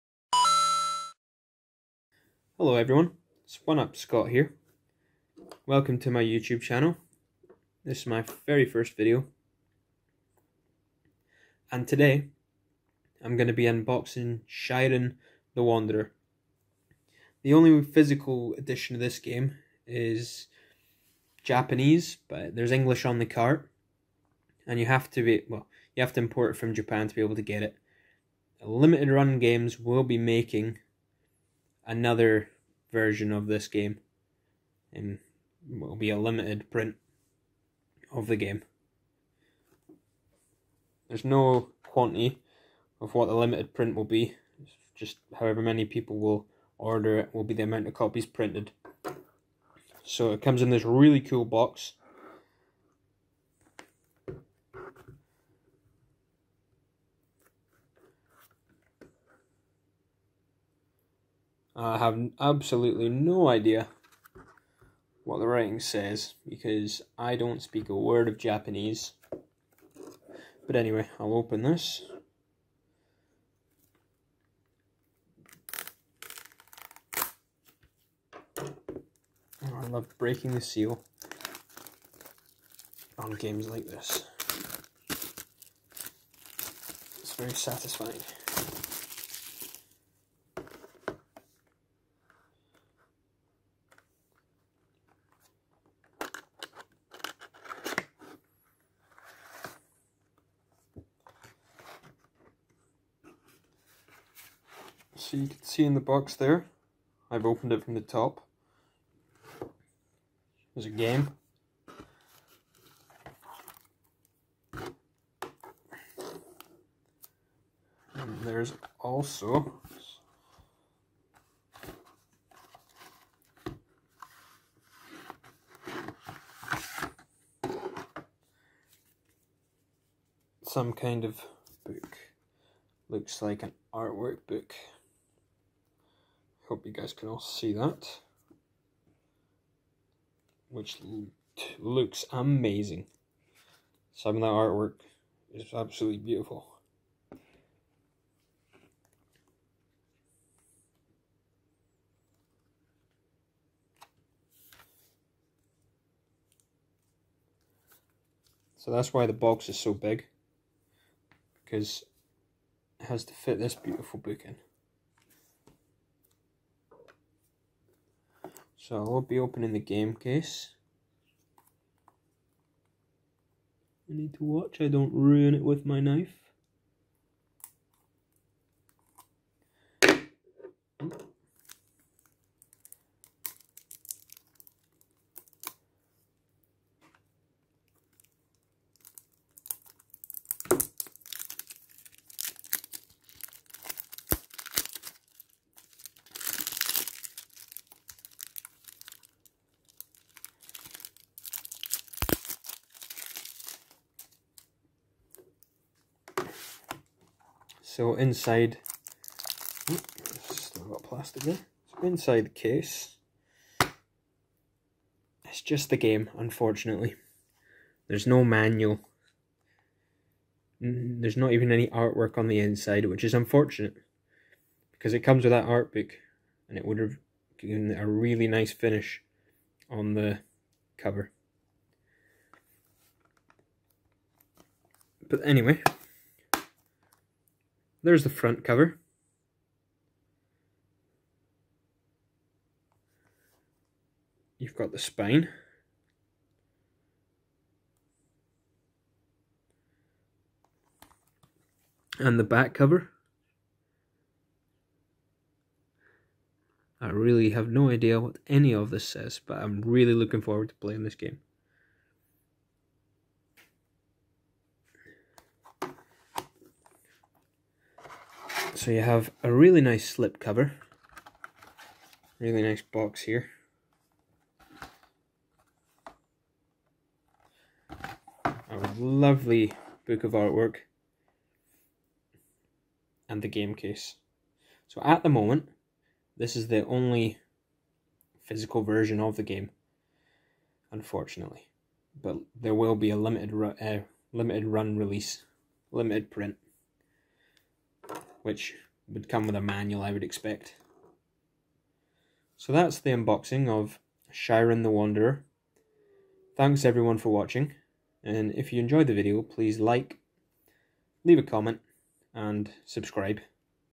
Nintendo Switch: Shiren The Wanderer Japanese Import Unboxing
after i recorded this limited run games announced their copy of Shiren!